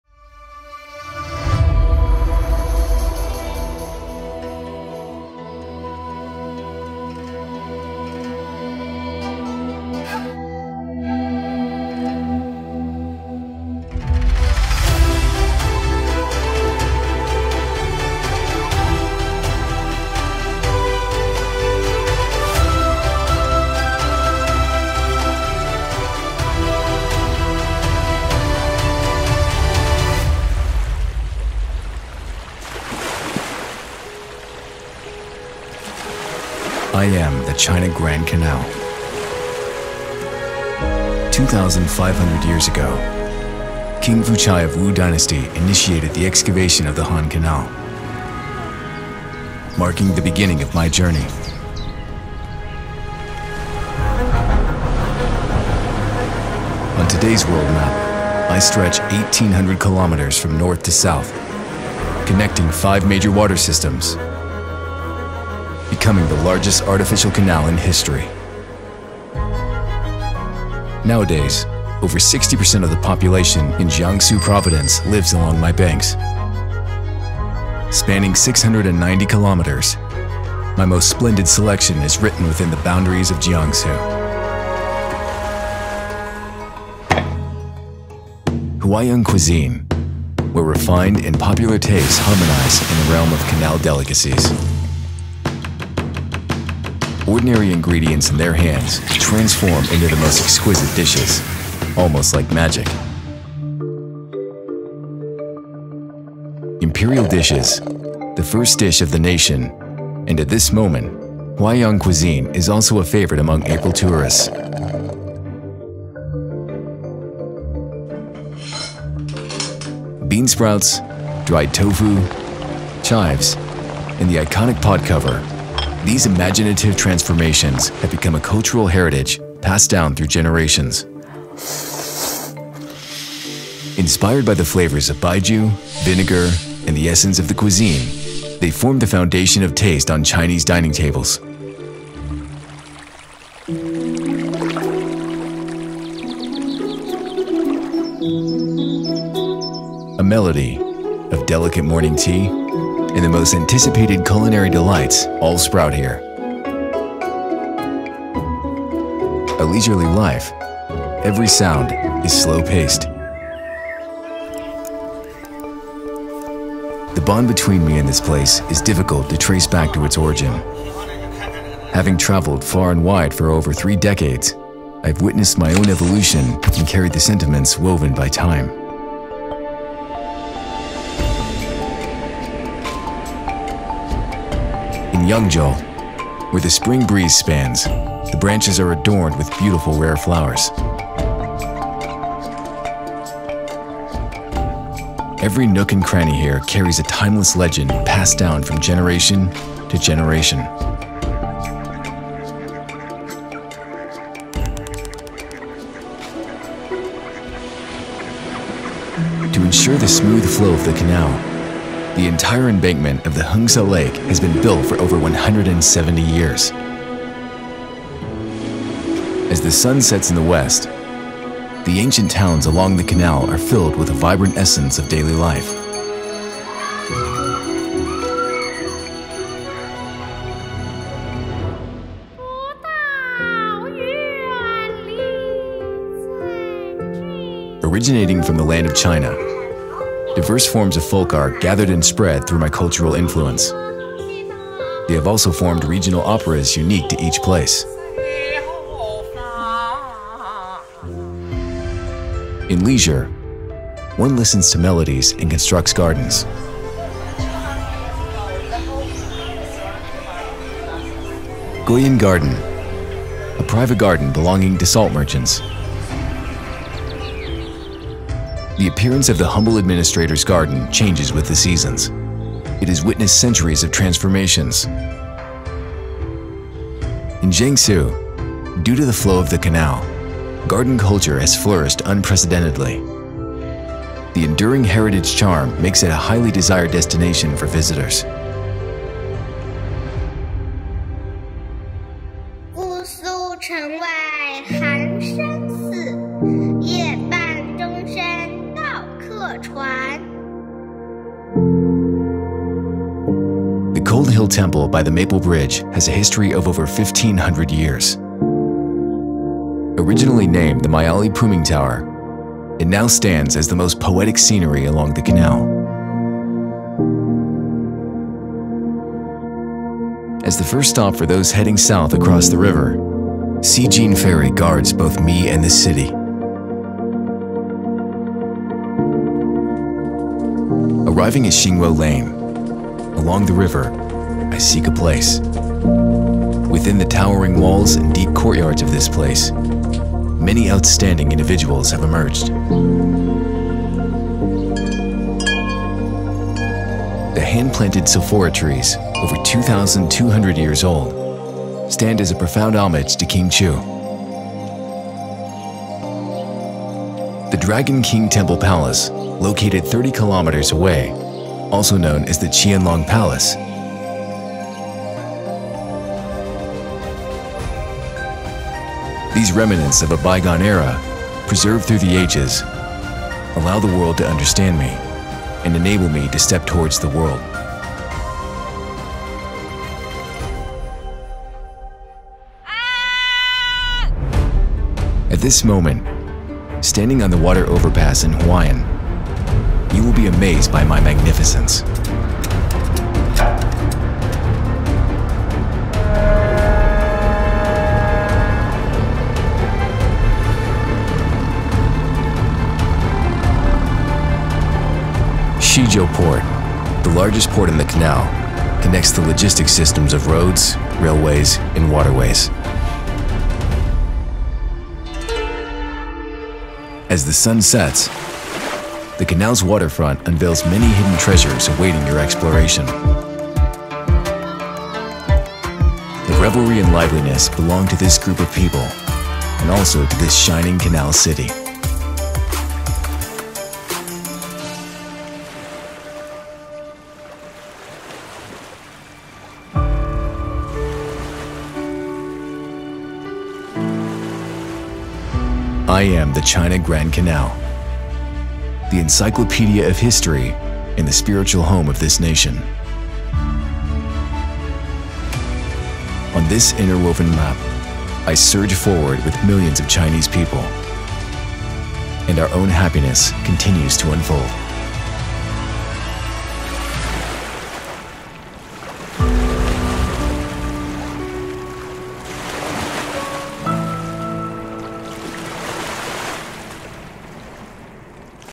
Nihao! China | The Flowing Civilization: The Jiangsu Section of the Grand Canal
The canal meanders leisurely, weaving its way gracefully. As a significant symbol of Chinese civilization, the Grand Canal, a waterway excavated by human hands, connects the vast water networks of the land of Huaxia, forming the economic and cultural lifeline of north and south China. Continuously, it writes magnificent poems of traditional culture and modern civilization on both banks. Along the Jiangsu section of the Grand Canal, 13 renowned historical and cultural cities and 28 national historical and cultural towns stand as a testament to its enduring impact. Its pulsating journey never ceases. Crossing millennia, it continues forward into the present!